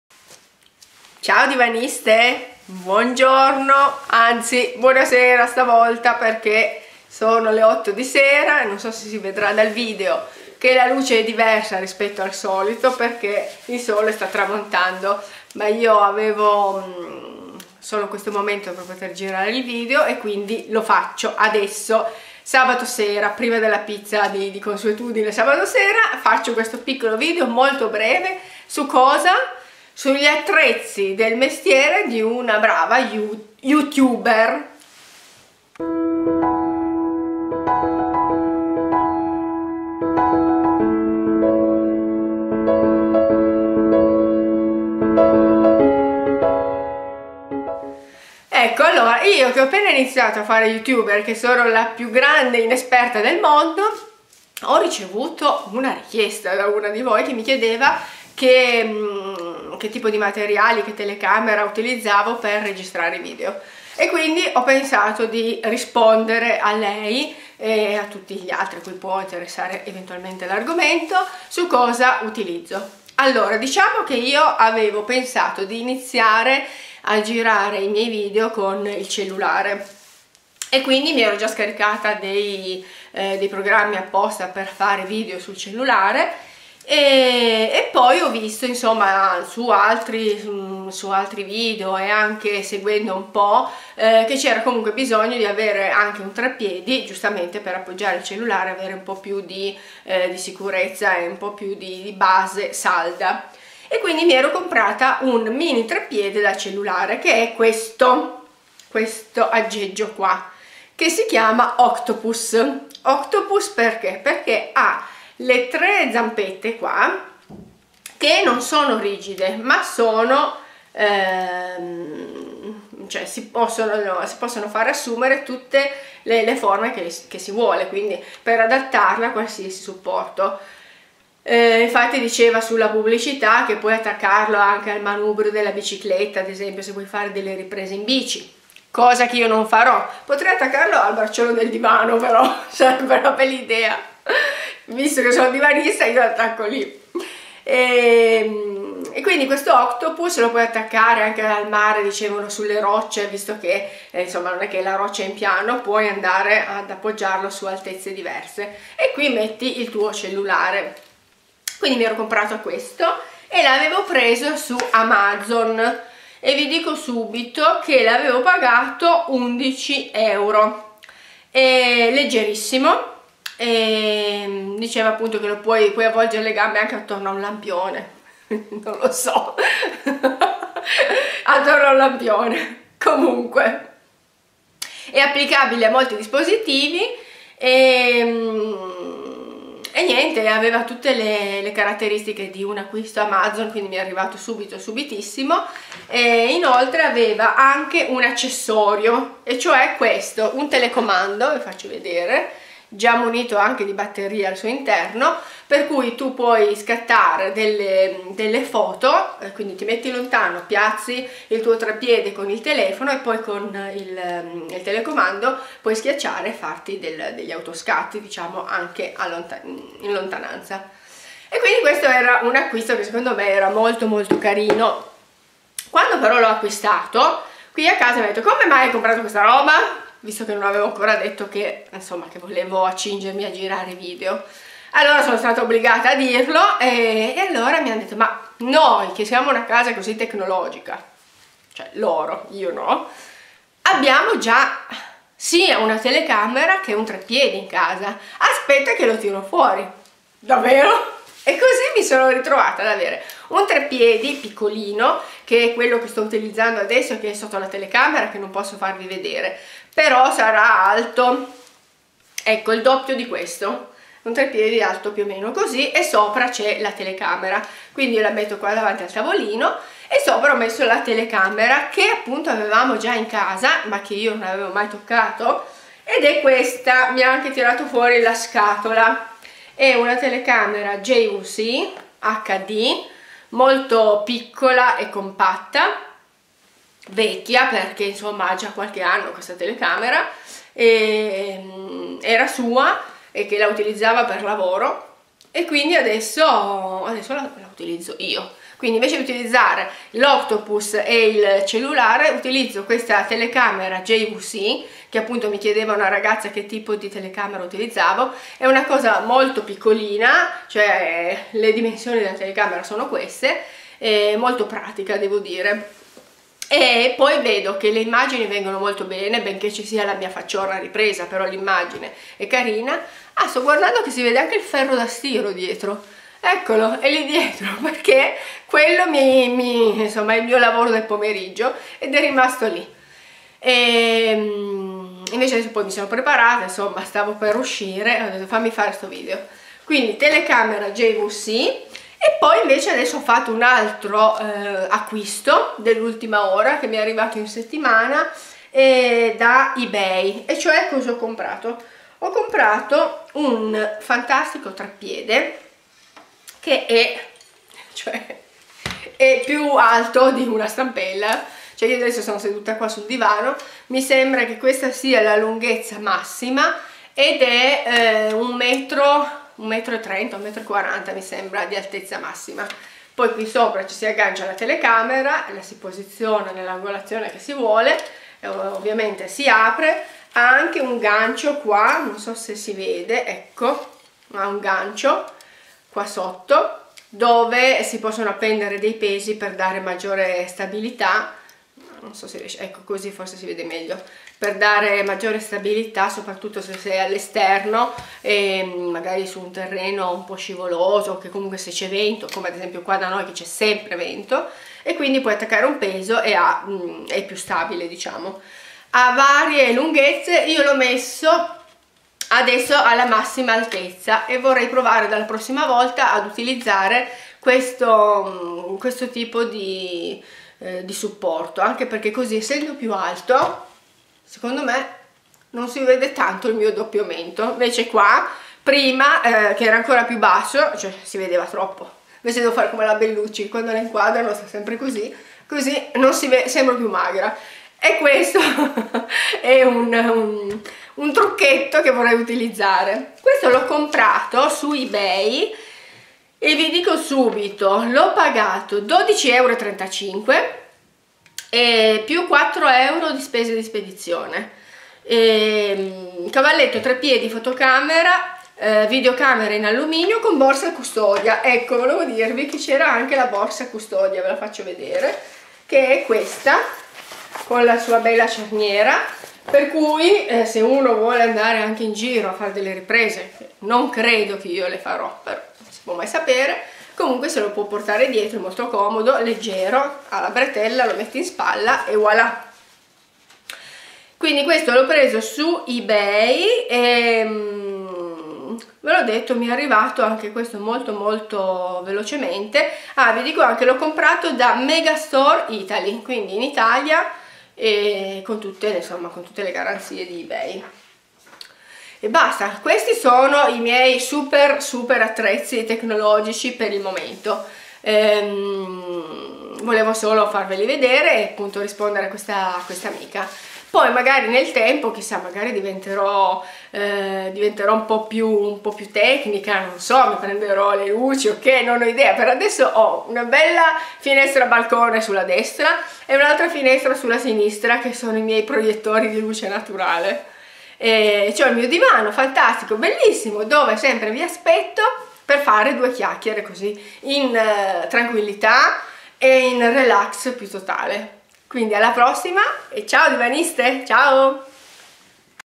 Cosa usi per girare i video per youtube?
O meglio: cosa adopero io per girare i miei video su youtube. Gli attrezzi iniziali ed indispensabili spiegati da una youtuber inesperta come me.